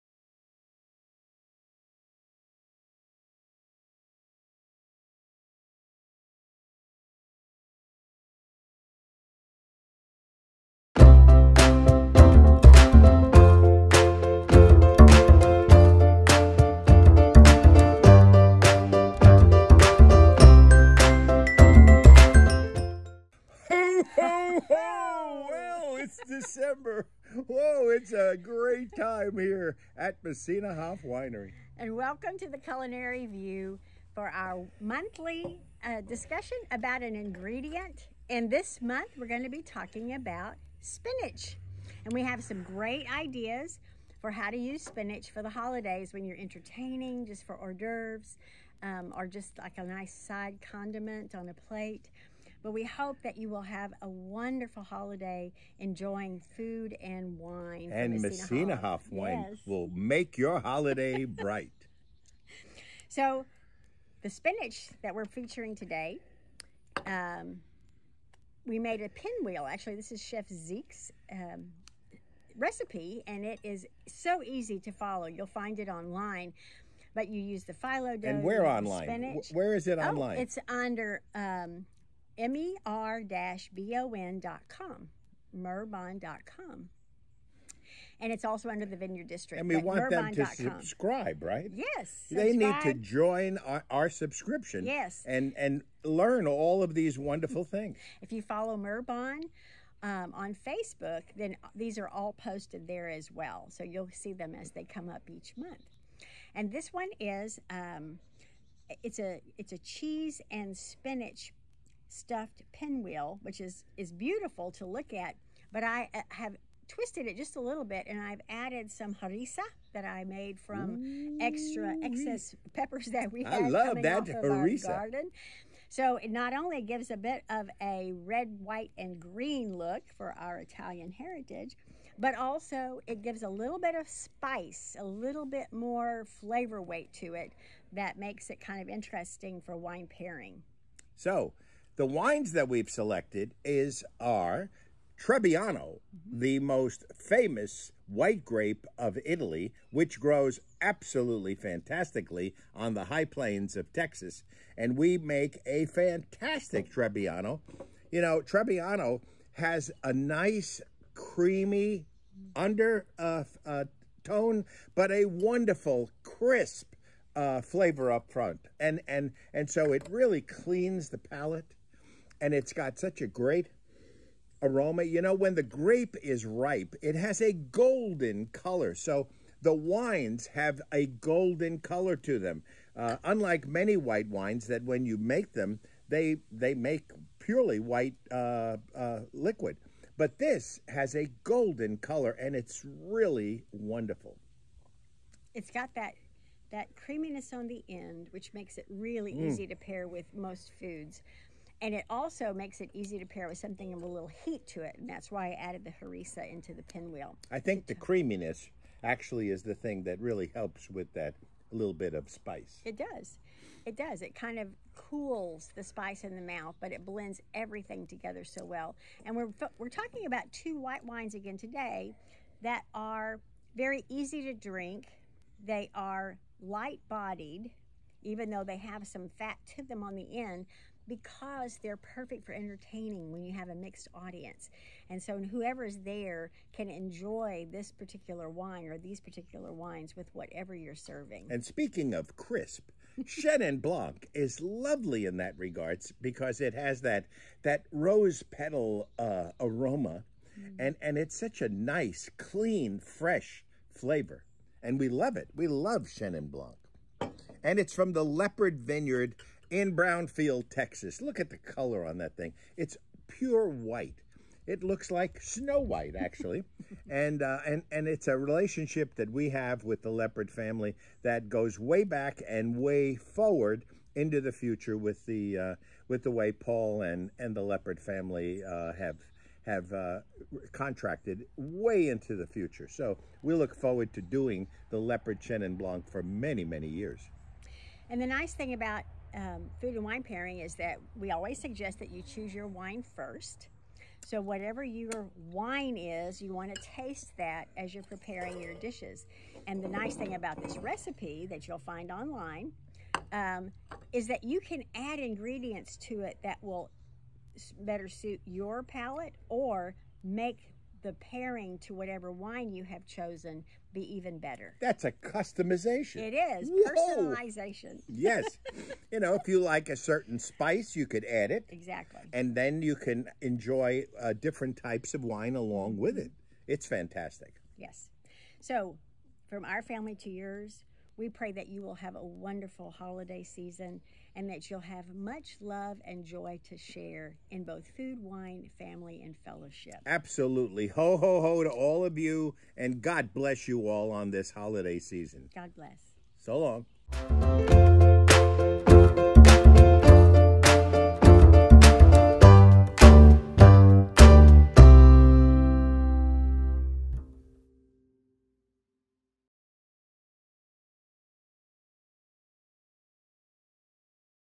Wine Pairing for Dishes with Spinach
Spinach has a delicate earthy flavor with a sweet finish. It is versatile to be used with red or white wines depending on other ingredients.